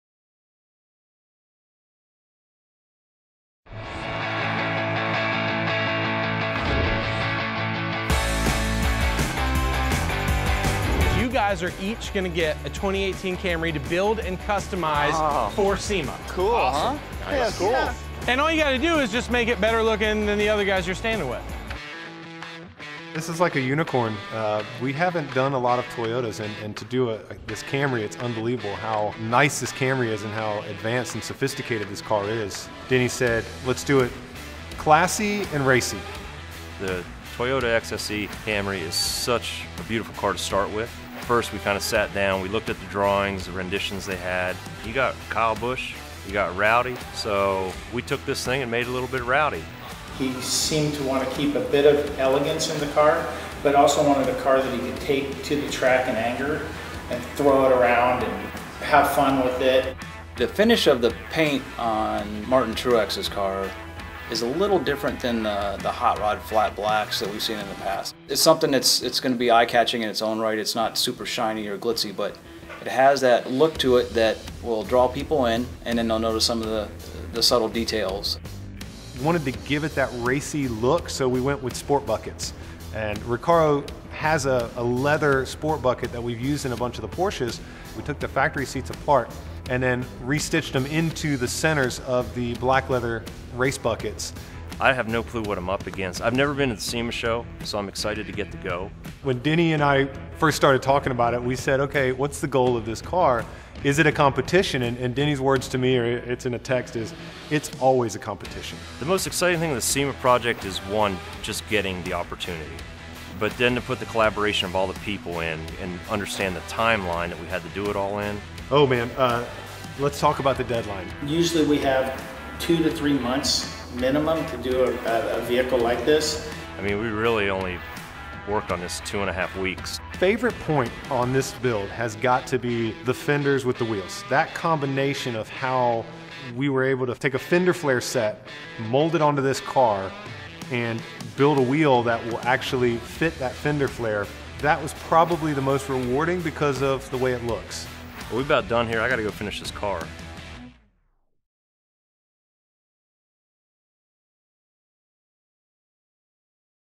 Toyota Camry SEMA Concepts by Martin Truex, Kyle Busch, Denny Hamlin & Daniel Suarez
Top NASCAR drivers Martin Truex Jr., Kyle Busch, Denny Hamlin, and Daniel Suarez brought their own spin on the all-new 2018 Camry to this year’s show. Each driver created a Camry that further elevates the all-new model’s cutting-edge design and enhanced performance.